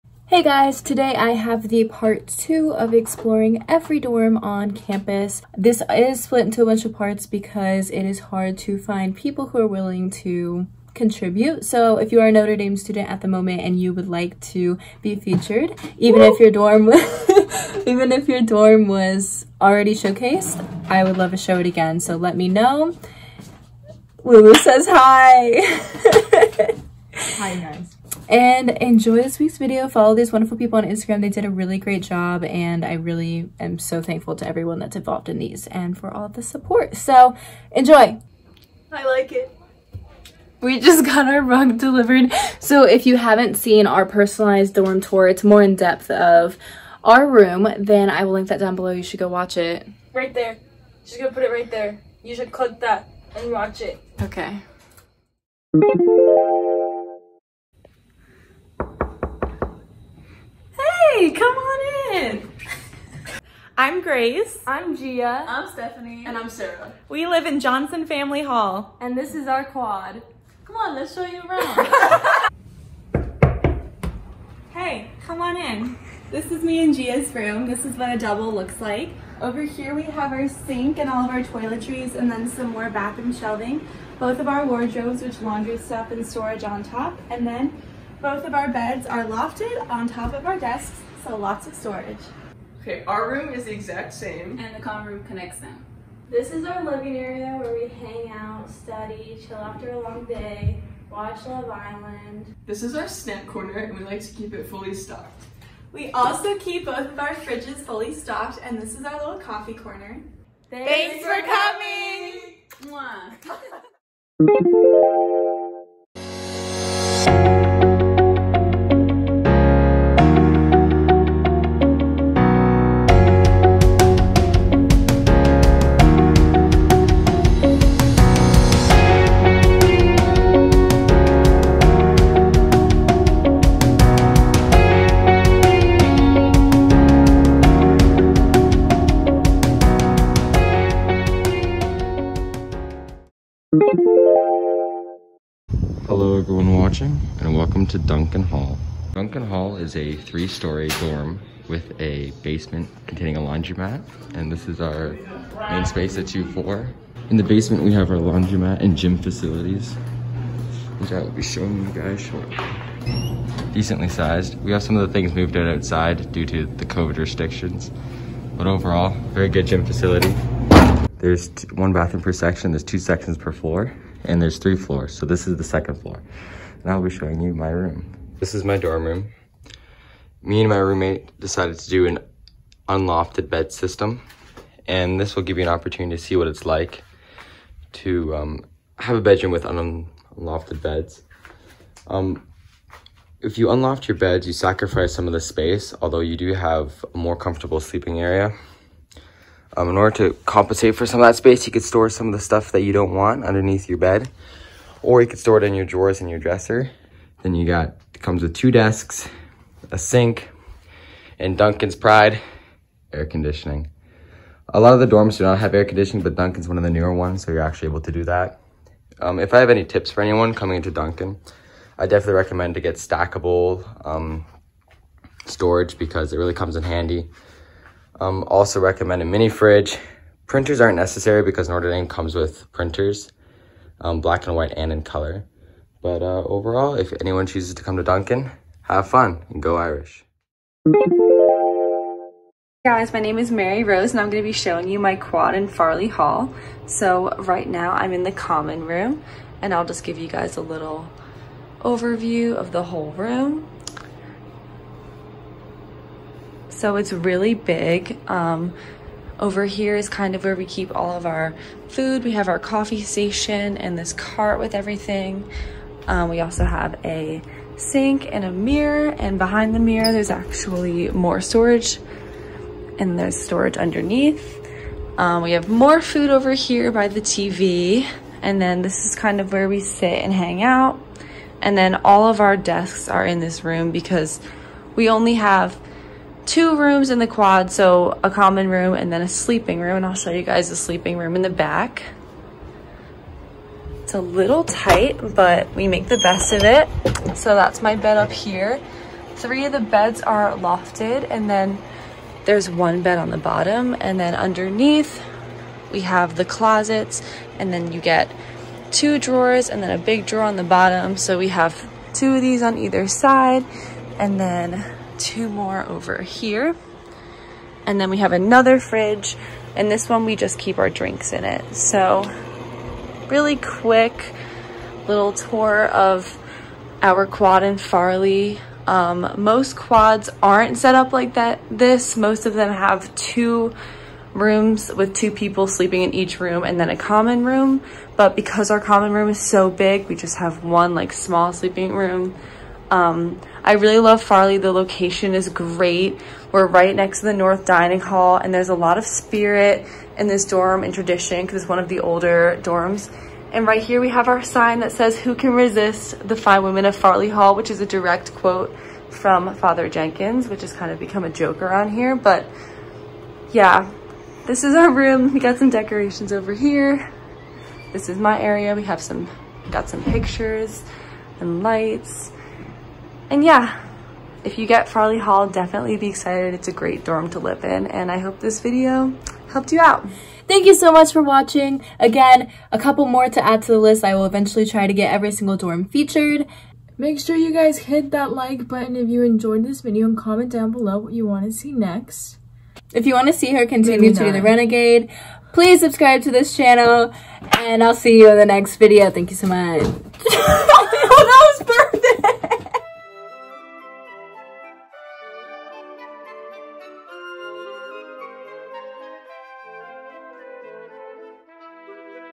Part Two: (not my) Notre Dame DORM TOUR ~ Dunne, Duncan, Farley, & MORE
HEY GUYS – By watching this video, you’ll see an example of a dorm room in female halls Johnson Family and Farley as well as male halls Dunne and Duncan! Which ones do you want to see next?!

Just because I live in the best dorm on campus, Lyon’s Hall, doesn’t mean I won’t try to give you all insight into other dorms, too!

Videos to look forward to!: Part 3 and more vlogs!